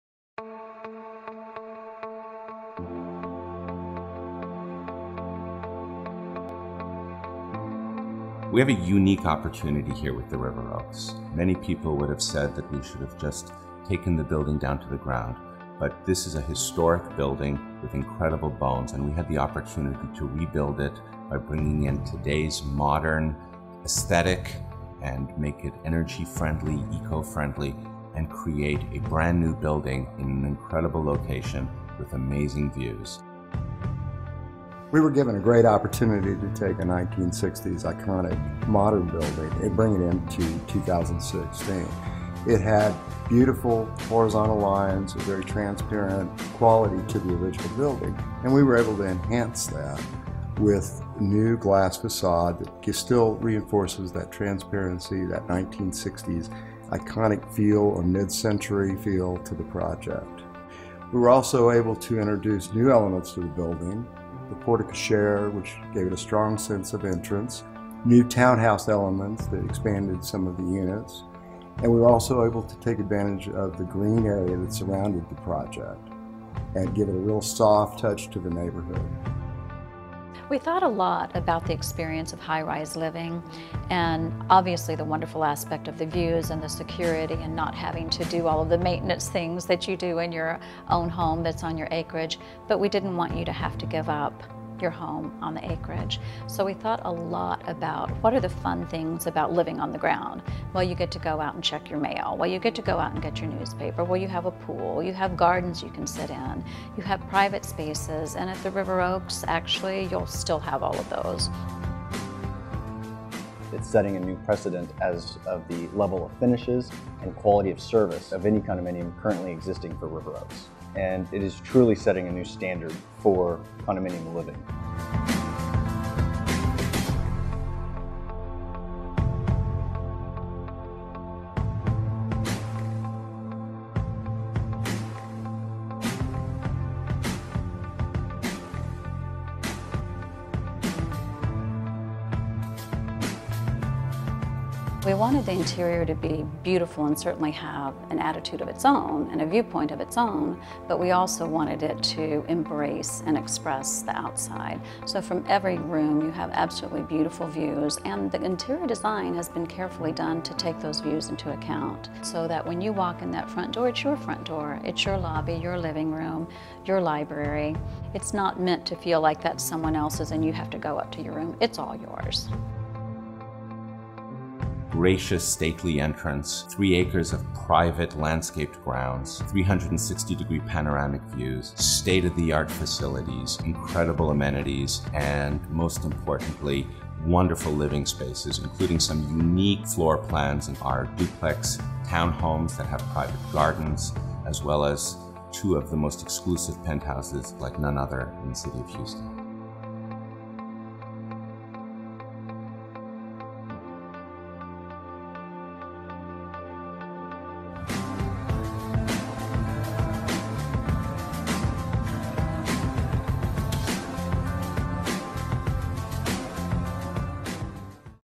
The River Oaks - River Oaks Story
The total transformation of The River Oaks into Houston’s most elegant and sophisticated vertical estate community is now well under way. This limited offering of only 79 generously sized residences is perfectly situated and surrounded by nearly three acres of beautifully landscaped grounds. The response has been extraordinary – The River Oaks is delighting the most discerning buyers seeking a private carefree lifestyle in the city’s most sought after neighborhood.